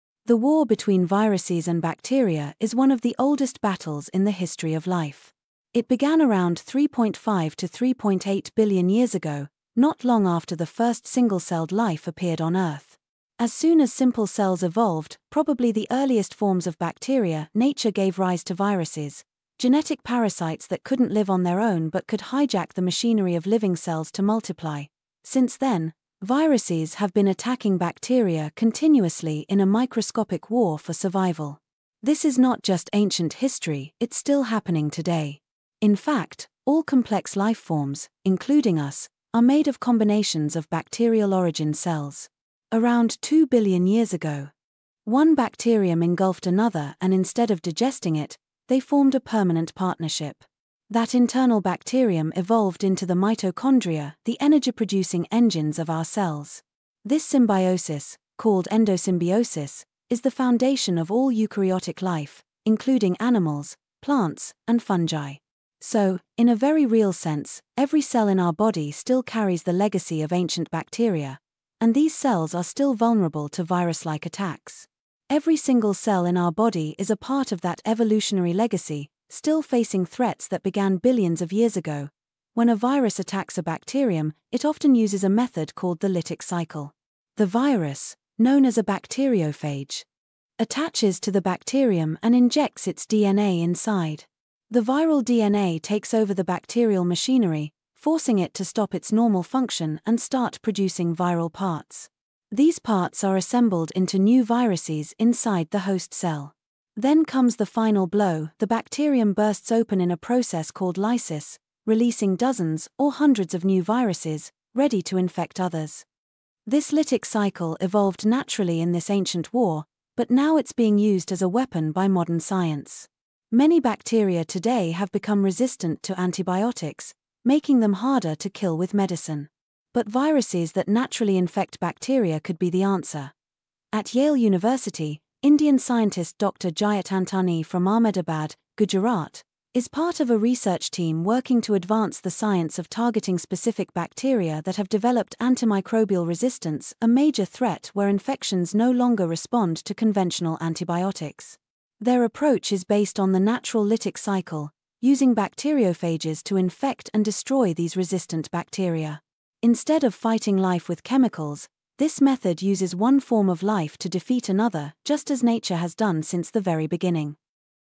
"Antibiotic Resistance's New Weapon: Viruses vs. Superbugs!"
Discover the epic, billions-year-old war between viruses and bacteria! 🧬 From the dawn of life, these microscopic entities have been locked in a battle for survival. Learn how this ancient conflict shaped all complex life, including us, through phenomena like endosymbiosis and the evolution of mitochondria in our cells.

But this isn't just history. Today, as antibiotic resistance becomes a global threat, scientists are turning back to nature's original warriors: viruses! See how bacteriophages—viruses that specifically attack bacteria—are being harnessed to fight deadly superbugs.

🔬 We explore groundbreaking research, including the work of Indian scientist Dr. Jyot Antani at Yale University. His team is advancing bacteriophage therapy, using the natural lytic cycle to target and destroy antibiotic-resistant bacteria. It's a revolutionary approach, fighting life with life, just as nature intended.